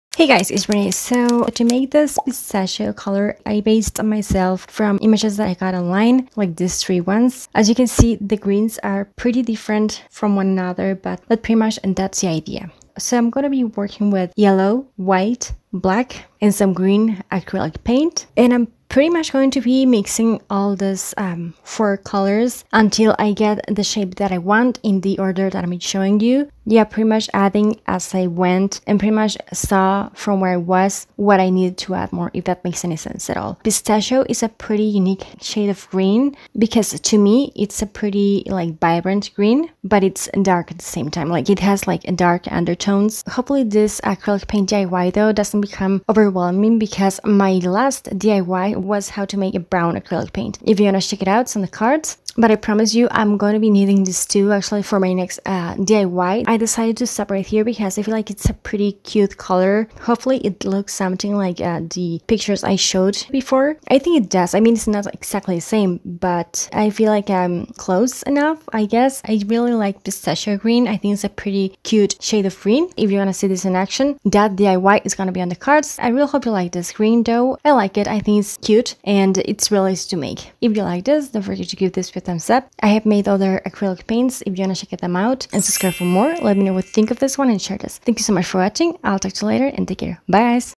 DIY Pistachio Color Acrylic Paint
Hi guys I hope you like this pistachio color acrylic paint, really easy to make!